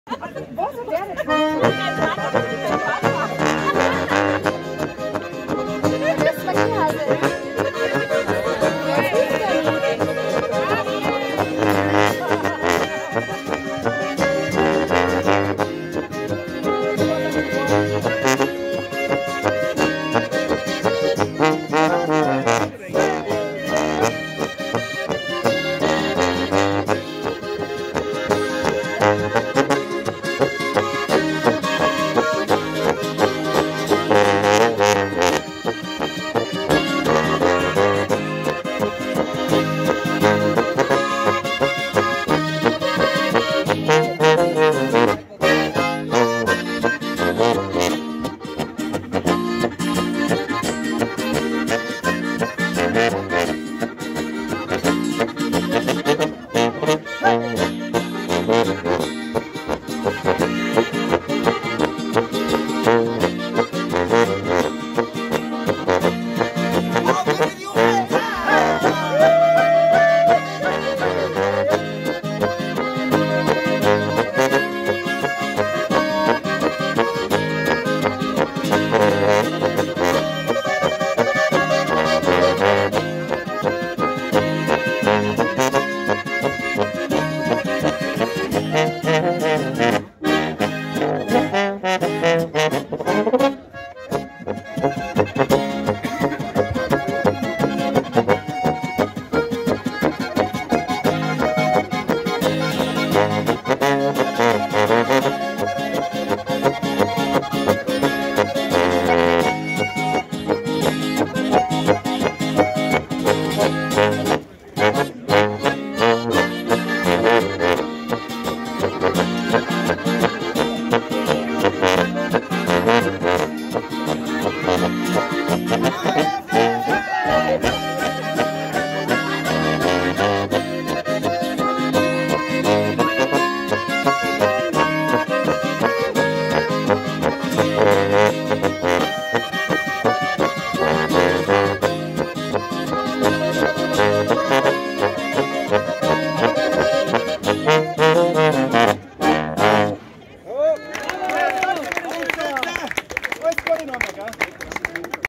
Musikfest - Zommhucka Musi -
Musikfest - Grazer Spatzen - Die Zommhucka Musi am Pfingstsonntag bei der alten Schmiede in Leogang.